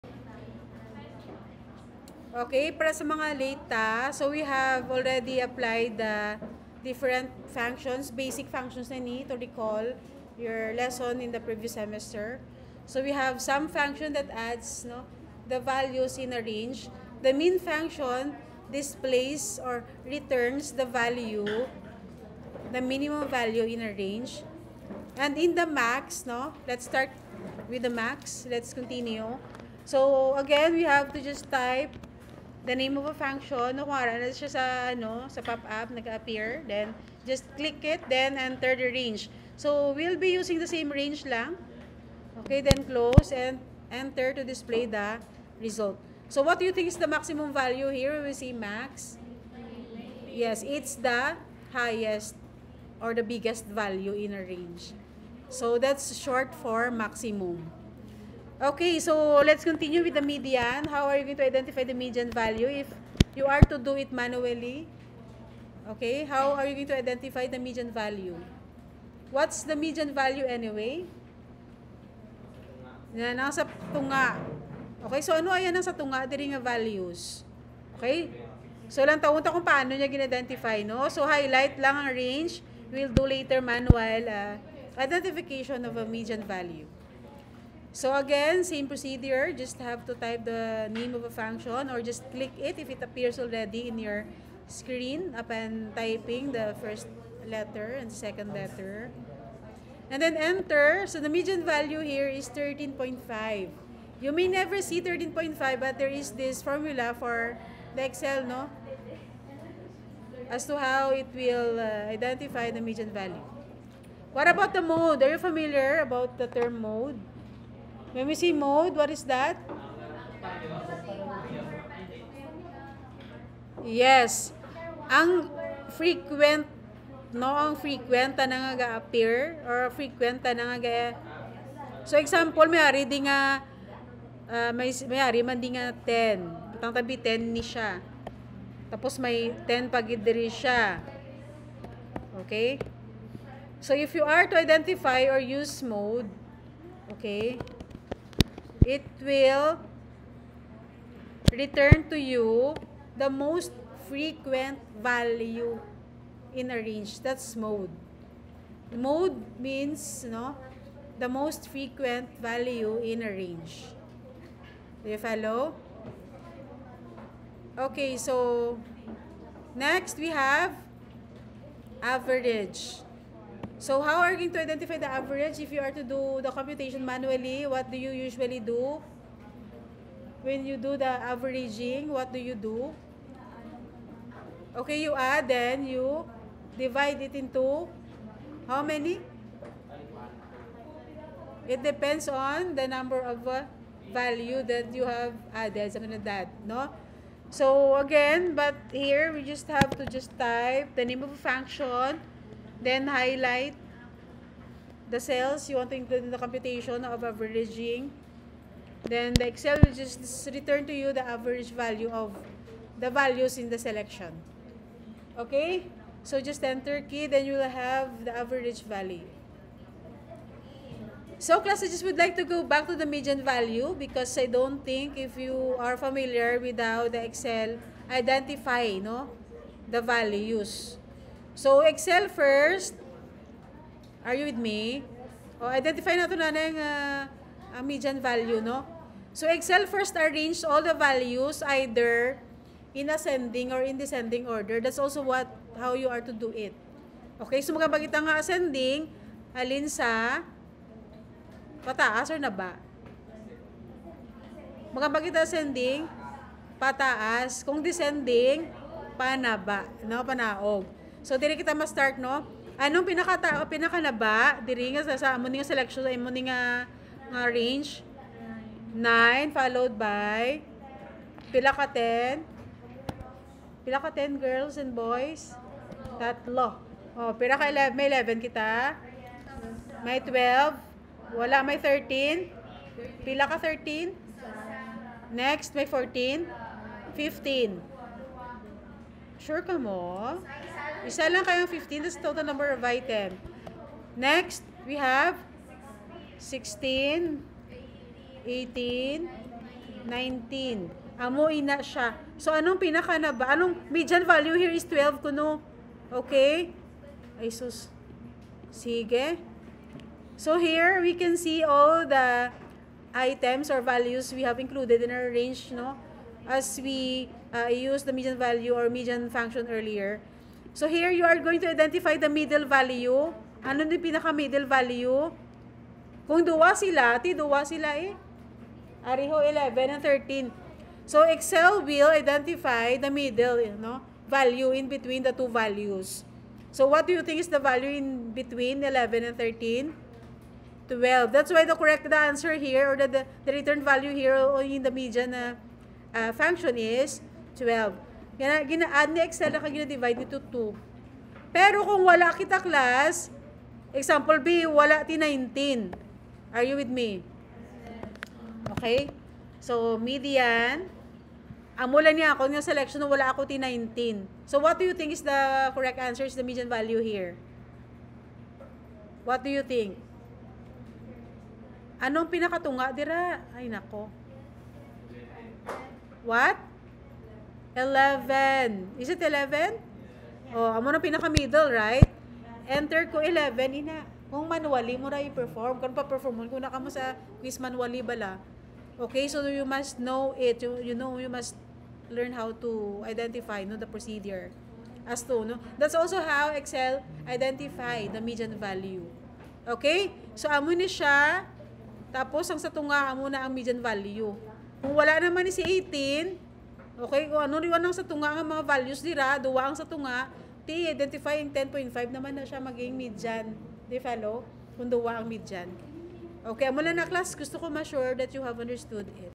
Basic Excel Functions
This video is an actual lecture during ICT class discussing basic Excel functions such as sum, min, max, median and mode.